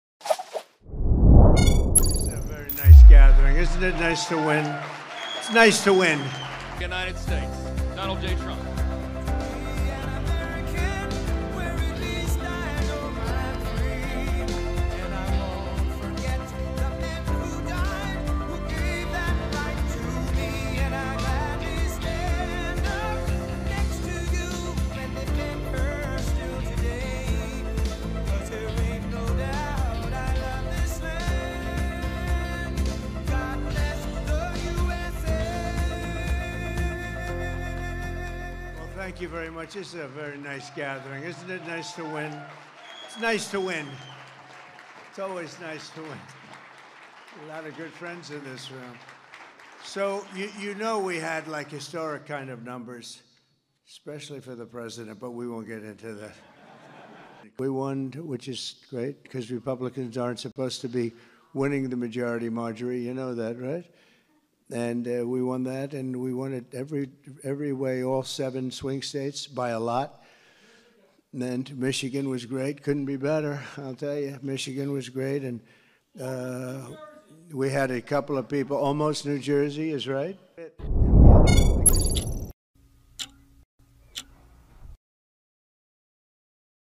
Donald Trump: Senang Rasanya Menang
Presiden terpilih AS Donald Trump bertemu para pejabat Partai Republik untuk pertama kalinya sesuai Pilpres AS. Trump merayakan kemenangan bersama partainya.